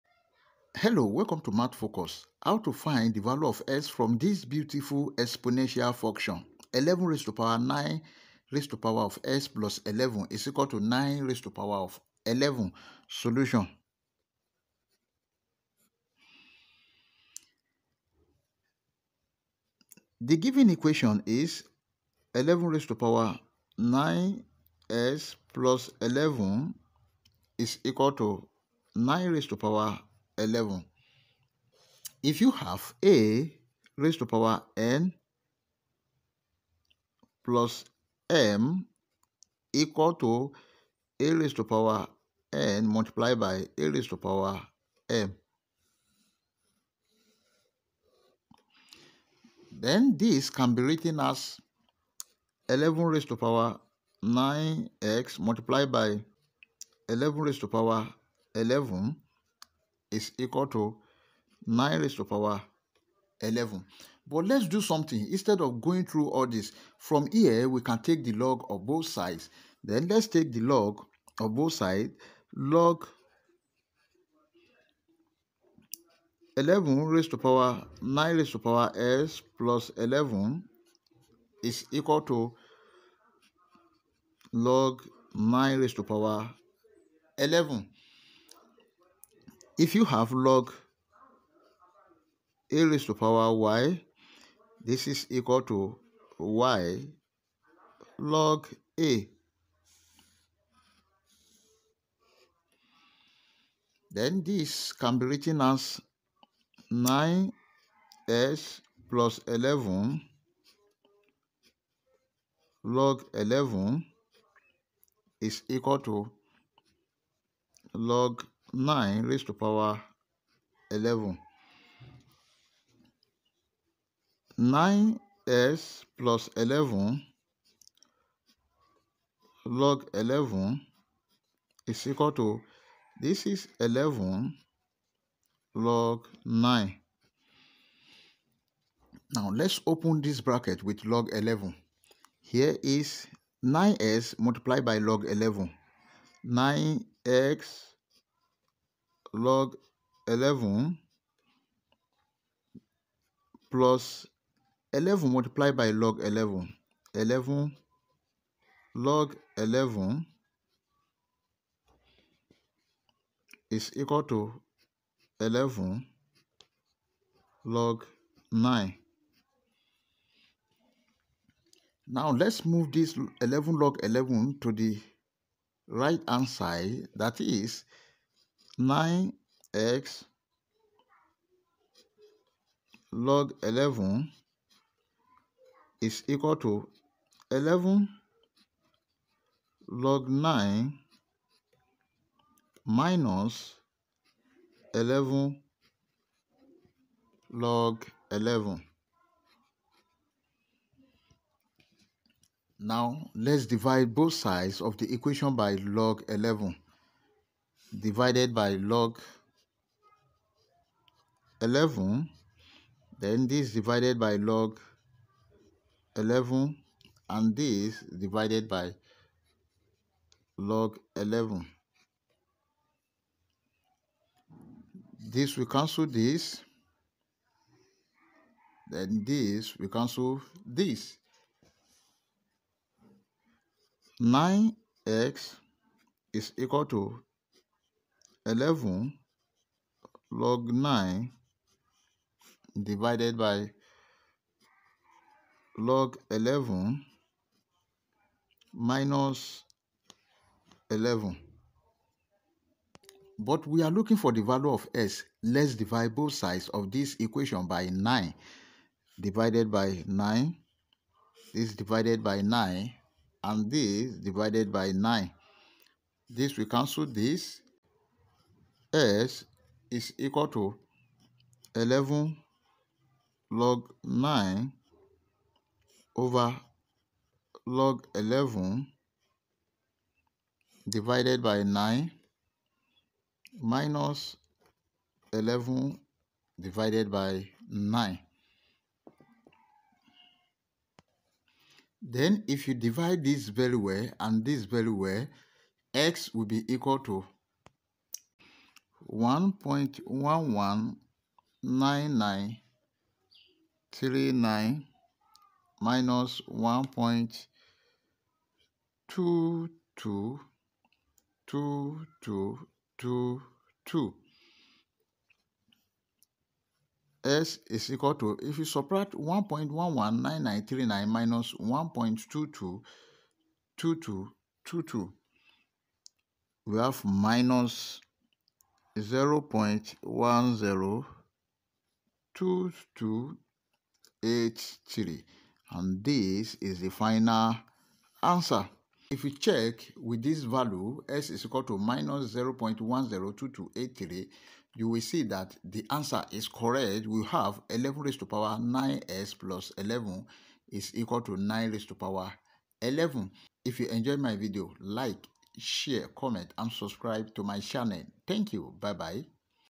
A Beautiful Math Problem 11^9x+11=9^11 | Math Olympiad Preparation | Exponential Function...
Hello Everyone👩‍❤️‍👨👩‍❤️‍👨👩‍❤️‍👨

 How To Find The Value Of X From This Math Problem.